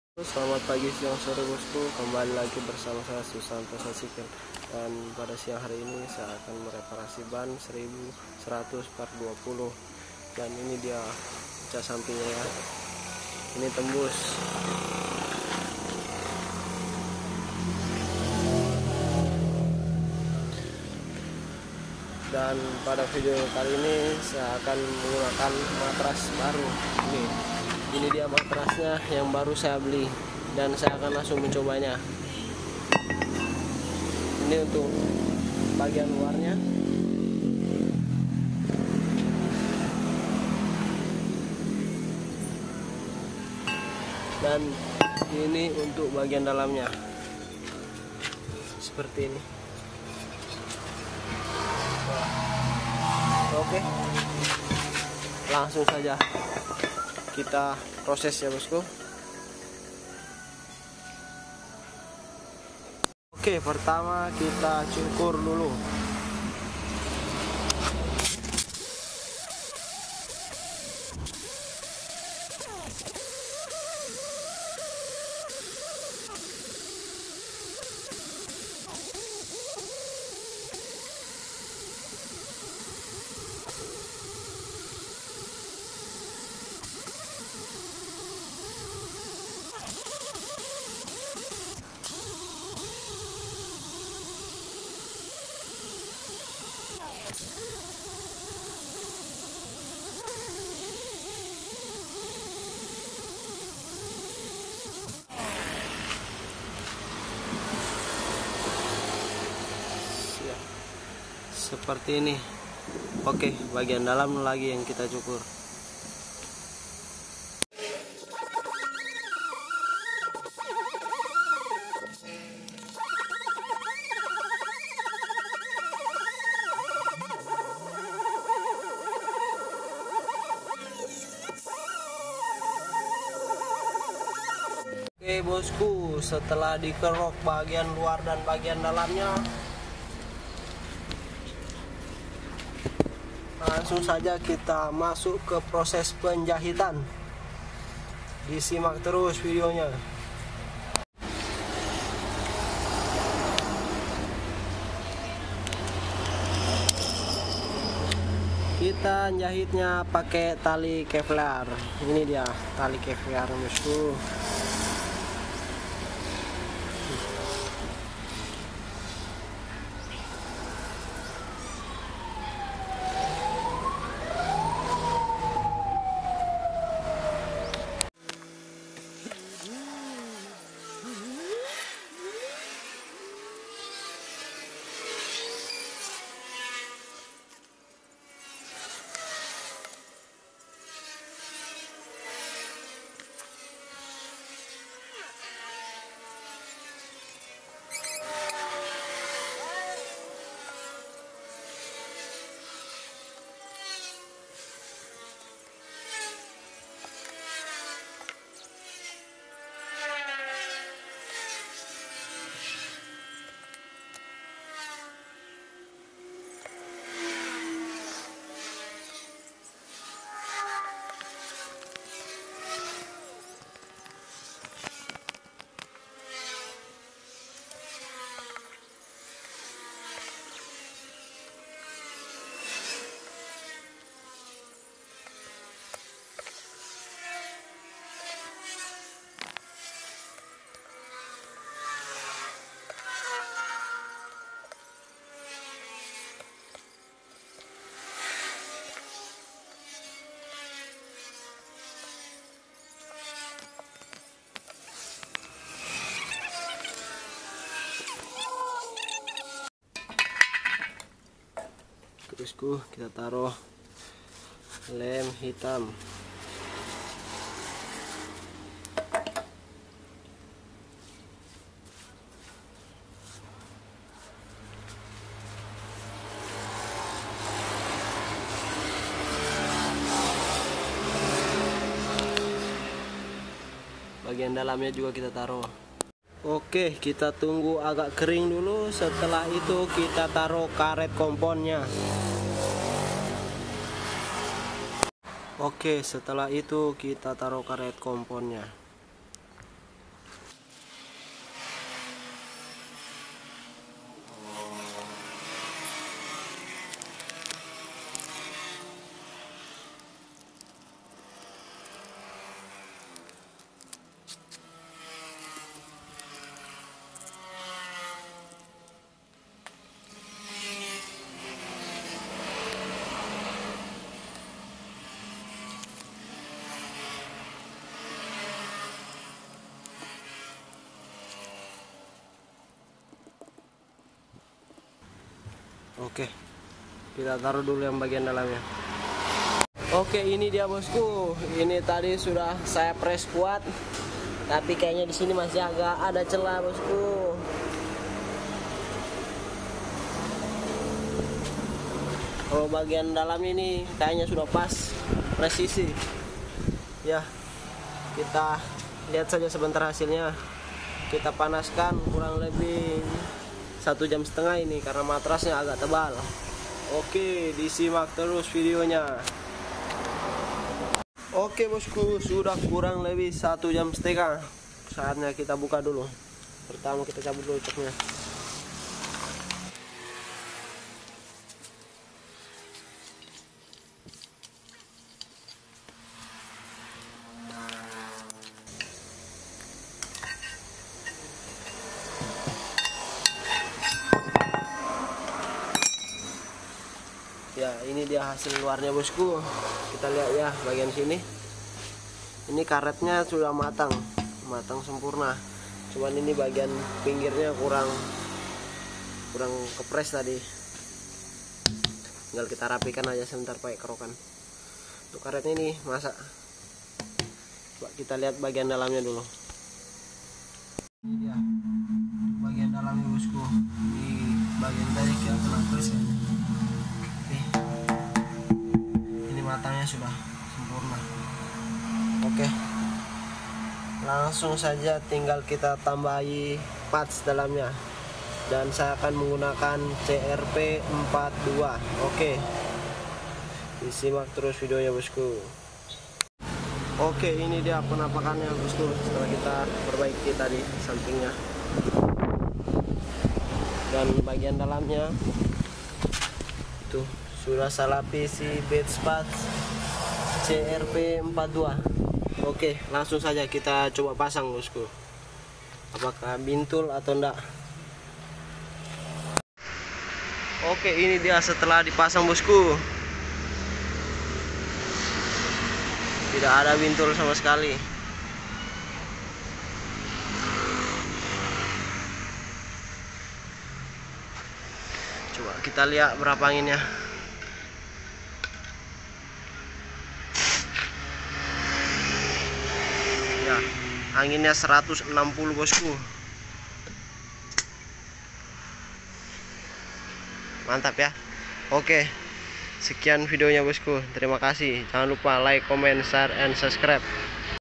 how to fix a side torn truck tire
perbaikan ban 1100/20
hotpres
bestpatch crp42
benang kevlar